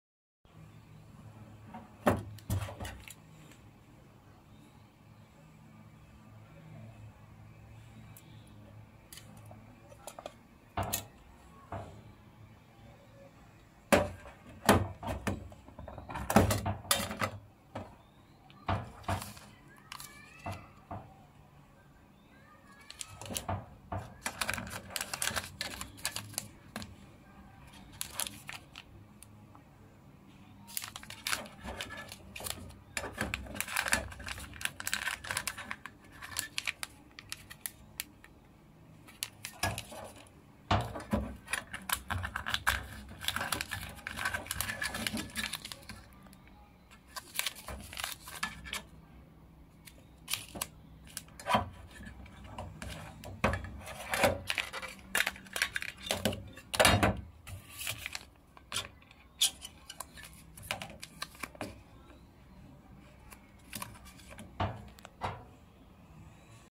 Twinings with THE MET Tea box. 川宁大都会联名茶盒.
茶盒内含有：豪门伯爵红茶X1包
英式早餐红茶X1包
精品锡兰红茶X1包
茉莉花茶X1包
蜜桃果香红茶X1包
The tea box contains:
English Breakfast Tea X1
Earl Grey X1
Finest Ceylon Tea X1
Jasmine Tea X1
Peach Tea X1